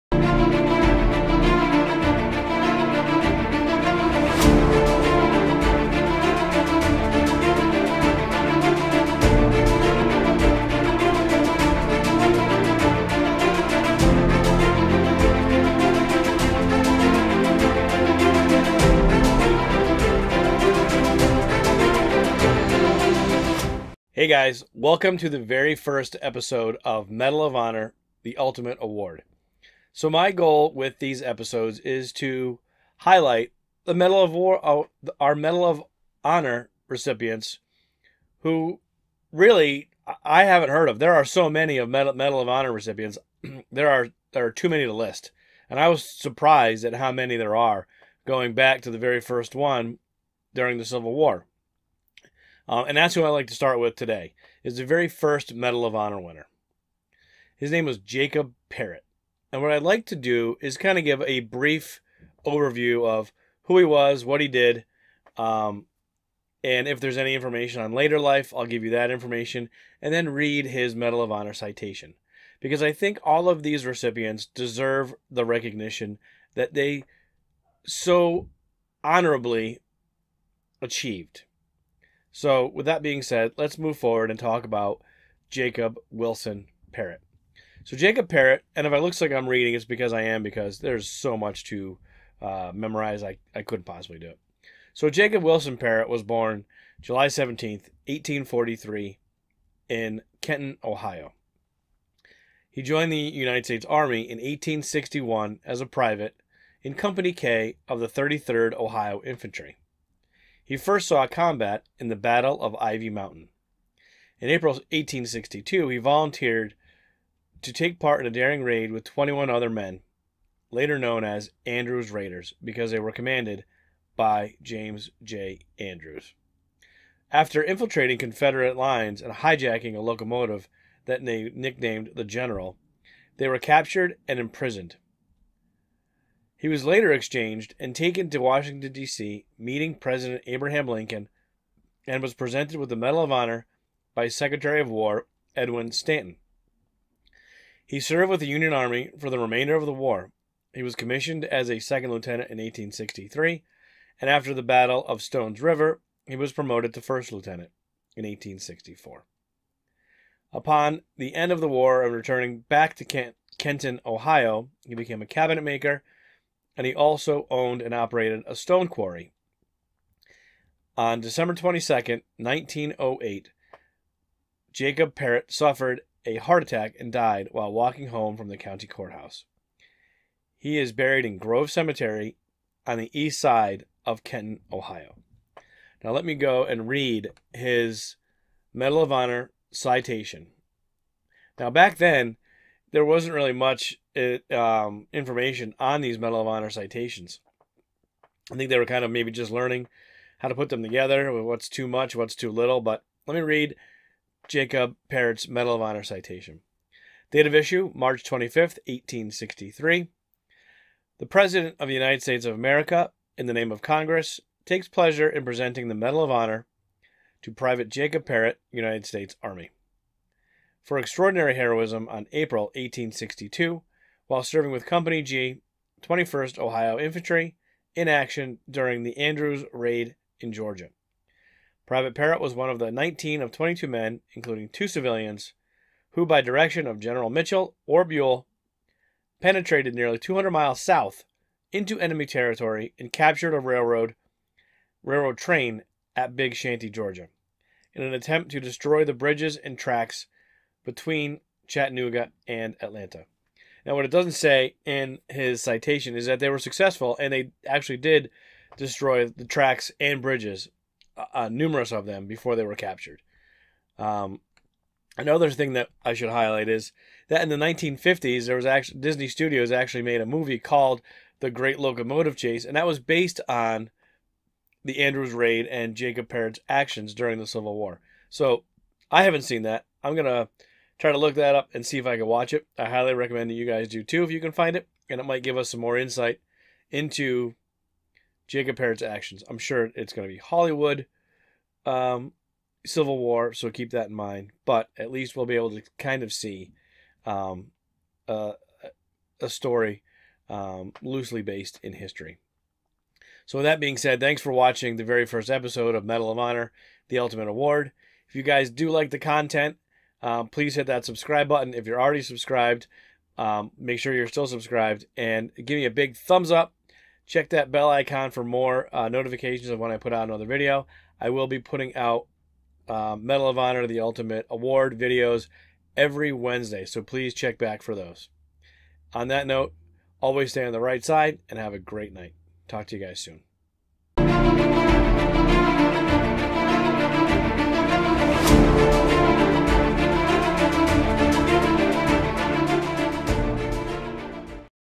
Medal of Honor, The Ultimate Award , Episode 1
Medal of Honor, The Ultimate Award, Episode 1.  Today I go all the way back to the first recipient of the Medal of Honor, Jacob Parrott in 1862.

vlogging gear: DJI Osmo pocket
                          Sony A6300